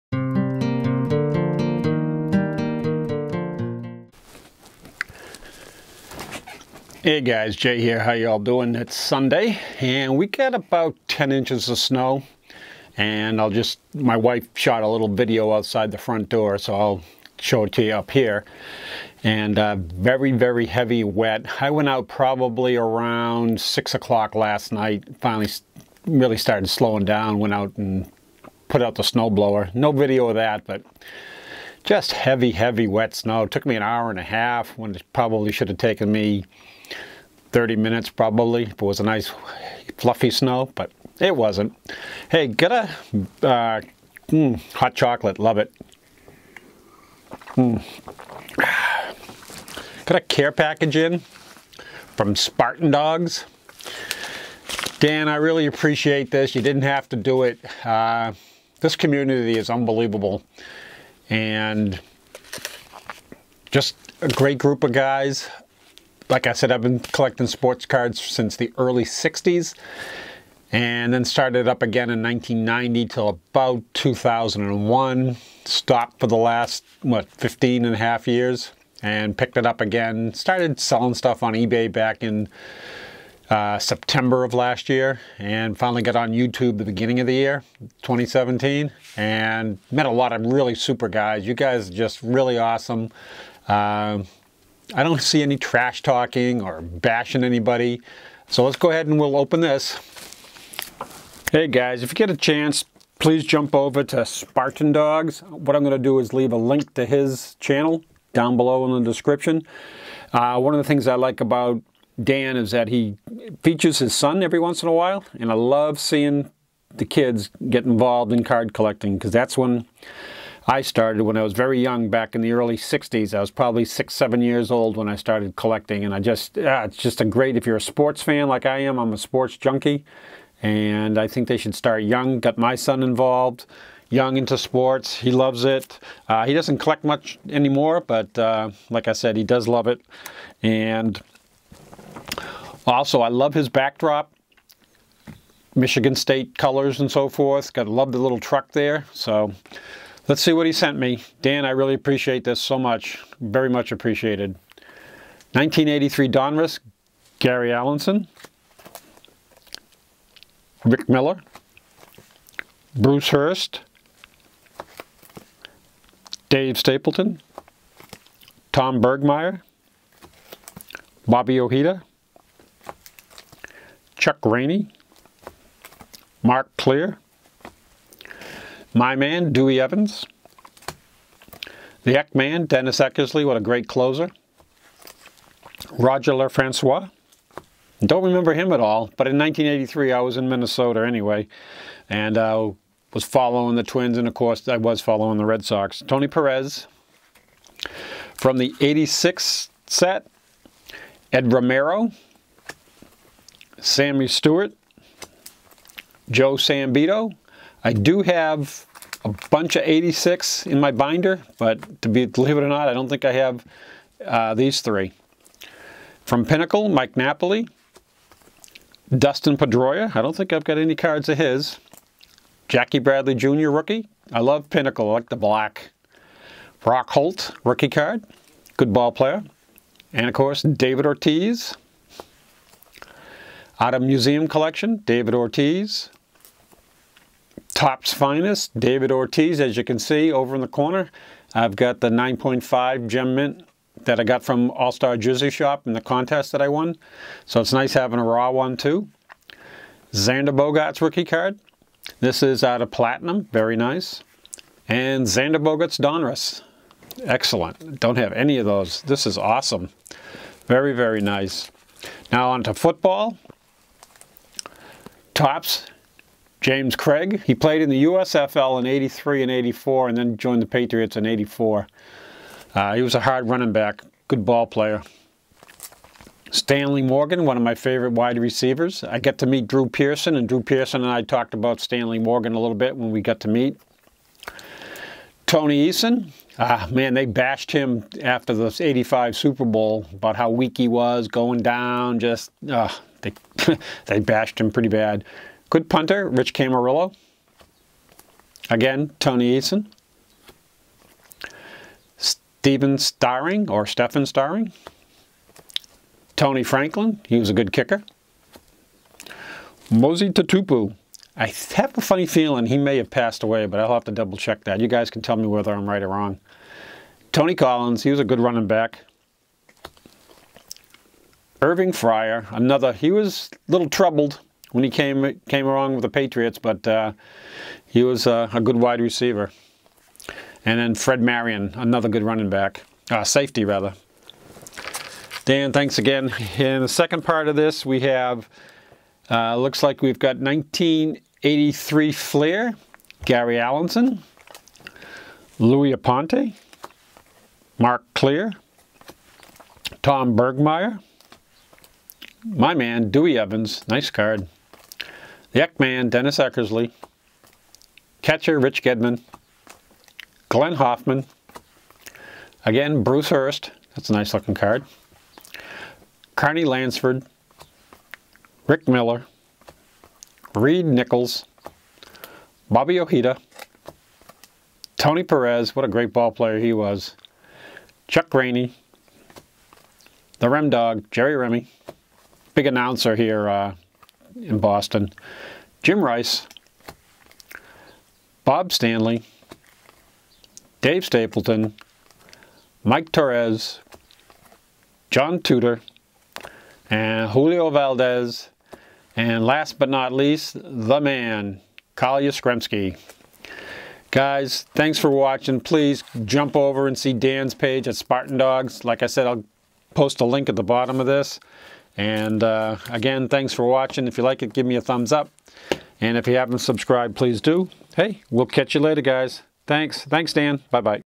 Spartan Dogs Care Package
Received a care package from Spartan Dogs, Dan Voigt. Red Sox and Patriots from the 80's & 2000's large package. Thank you so much Dan.

Jay McGillicuddy
Berkshire Hathaway Verani Realty